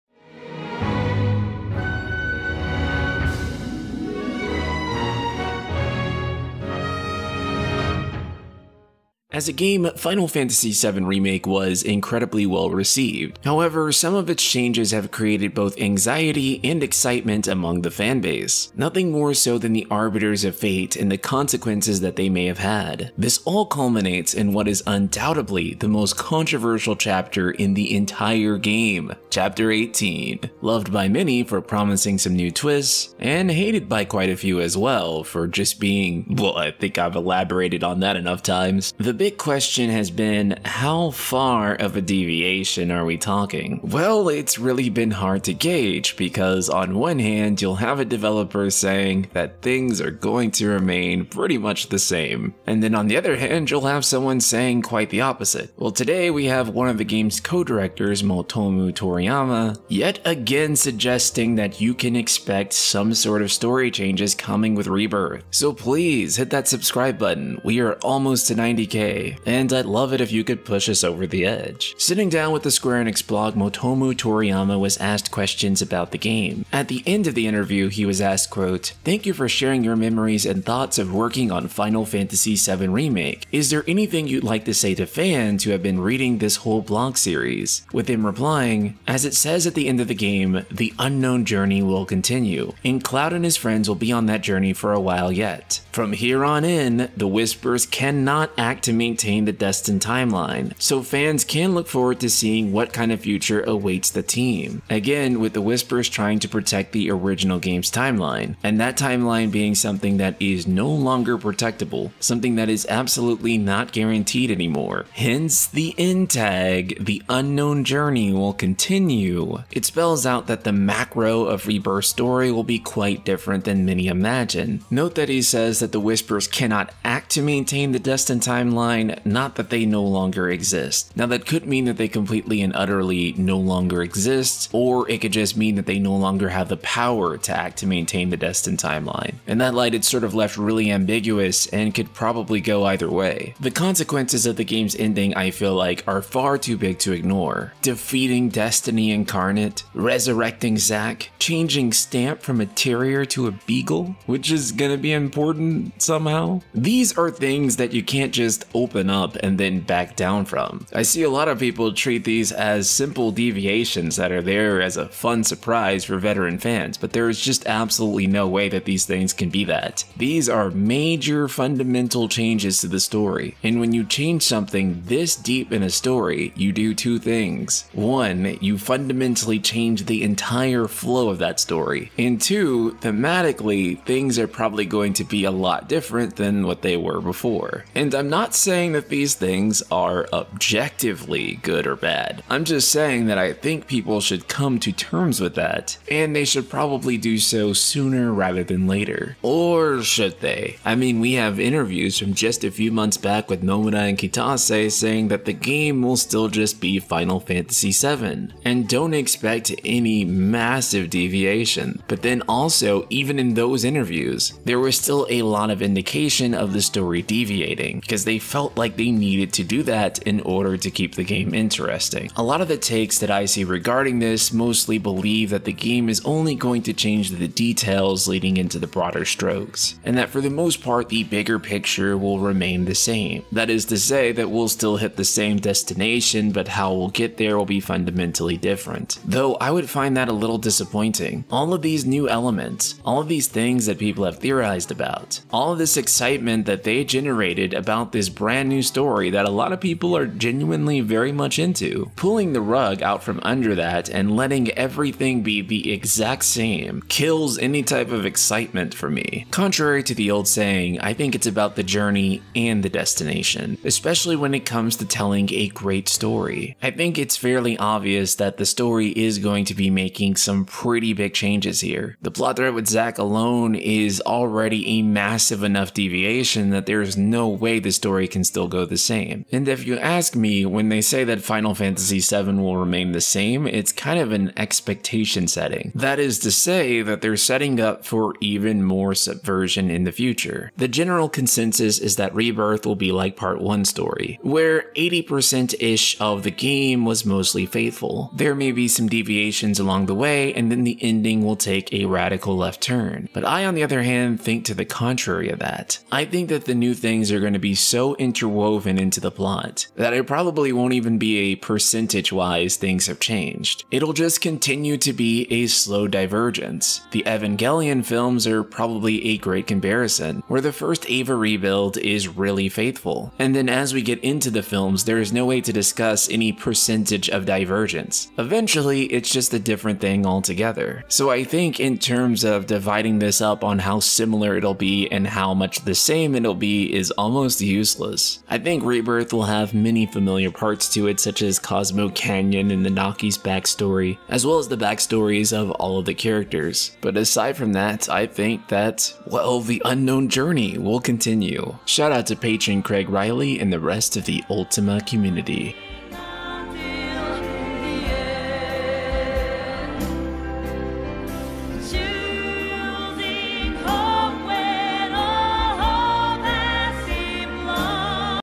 Final Fantasy 7 Remake Dev Teases Story Changes To Come in Rebirth
Final Fantasy VII Remake's Motomu Toriyama has stated with a lack of Whispers, that Destiny (OG's timeline/story) is no longer guaranteed to happen moving forward.